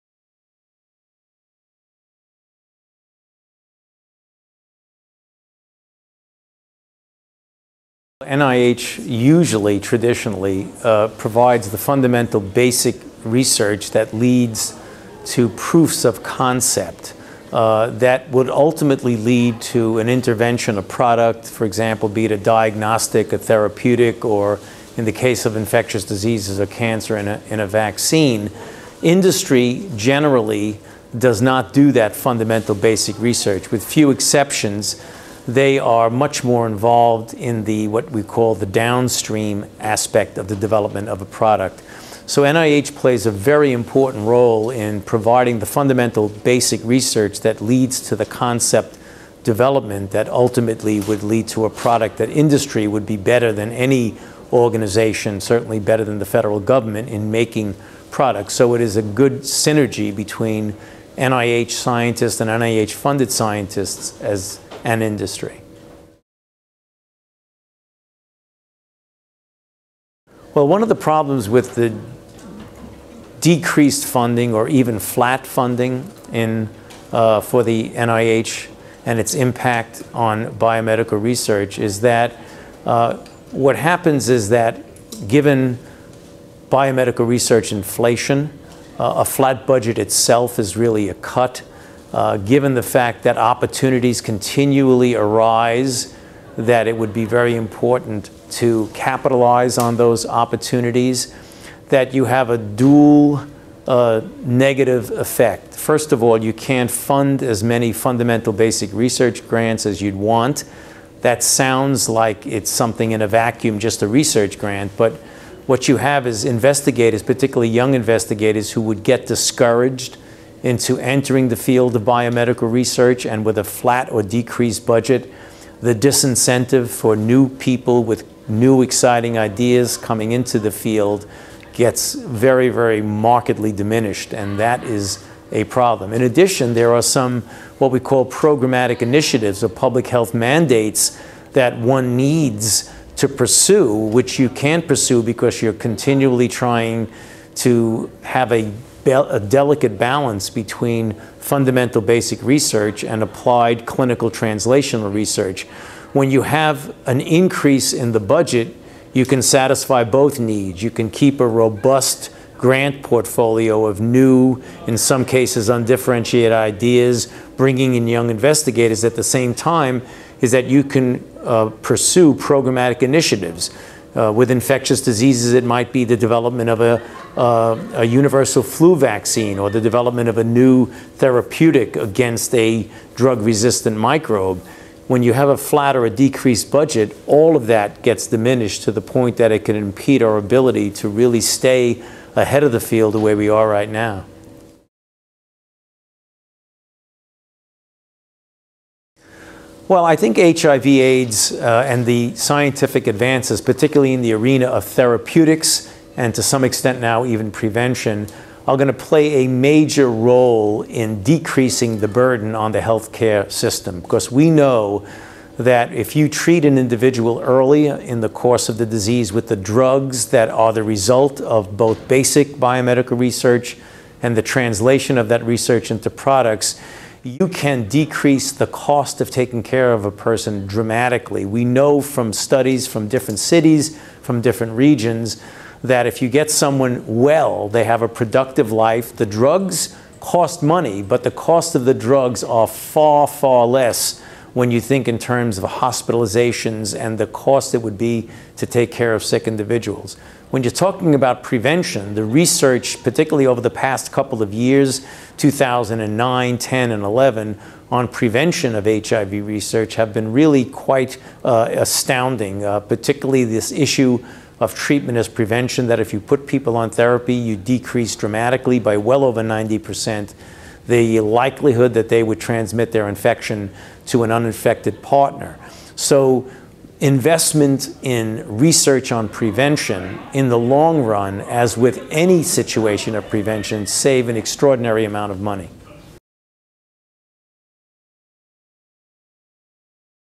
Interview with Anthony Fauci M.D.: The Value & Impact of NIH Funded Research
In a Dec. 5 briefing, Anthony Fauci, M.D., director of the National Institutes of Health (NIH)'s National Institute of Allergy and Infectious Diseases (NIAID), updated congressional staff and other participants on the role NIH and NIAID have played in addressing health challenges such as HIV/AIDS and influenza.